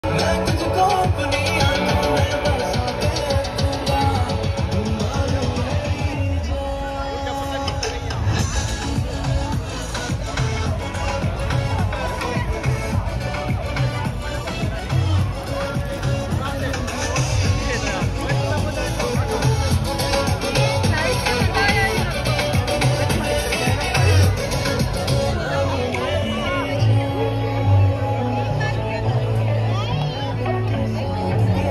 Kite Festival 2023 | 4K | Castle Hill Showground | Sydney | NSW | Australia
Glimpse of Kite festival 2023 held on 15th Jan at Castle Hill Showground
Follow @journeyoflifewithroy to see more videos like these.